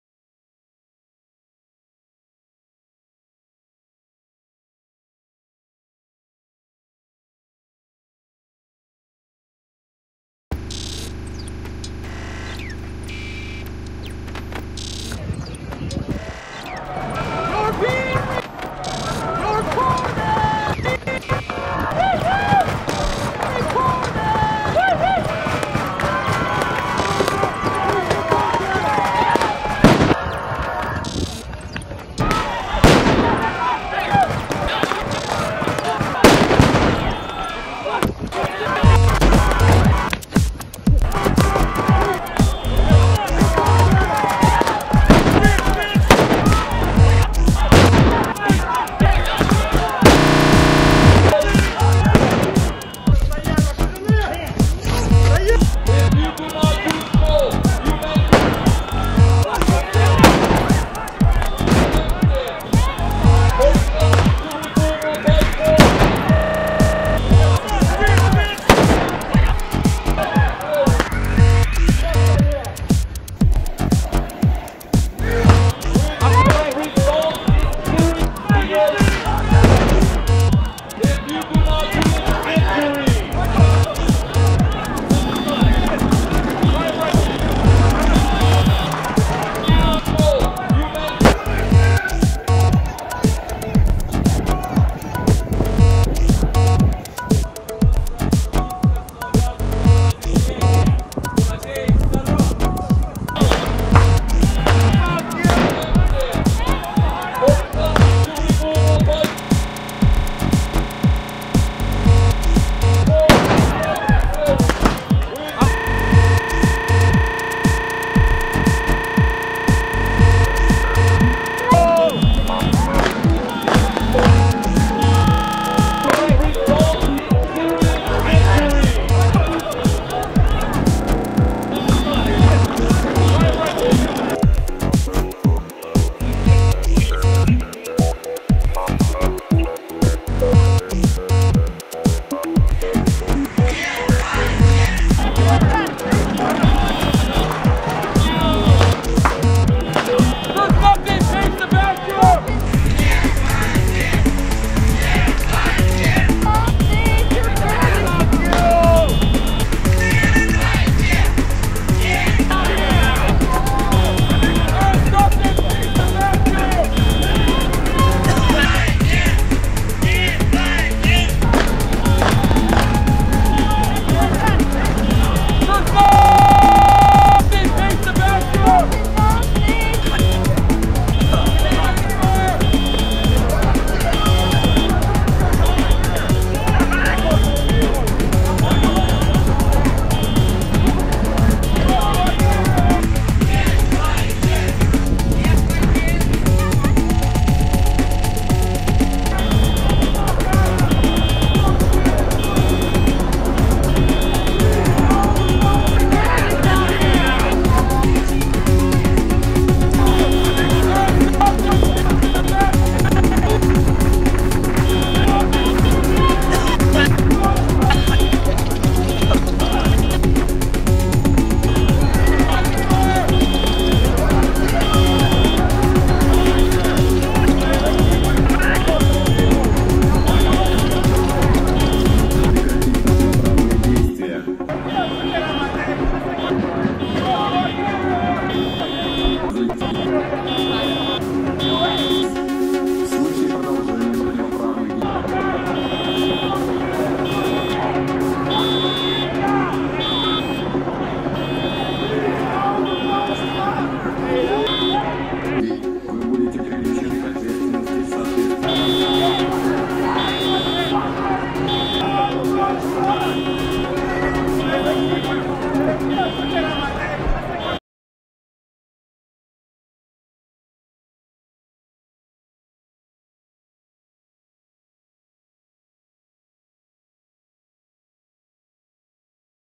supermächte
this video is dedicated to two movements that have changed lives in the countries in which they operate. it is occupy oakland, whose protest led to significant changes in the life of the united states, and the anti-war movement in russia, showing that not everyone supports the war in ukraine, and there are many who oppose it. both the usa and russia are so-called "superpowers" pursuing their imperial-capitalist ambitions on the world stage. but even such strong states tremble with fear when the propaganda of those in power does not work and the population goes out to protest and get their way. these movements must live now to fight the capitalist crap that is corrupting the world.

video: irick neumann
           helmut landers

audio: irick neumann
            helmut landers

made in apple motion 5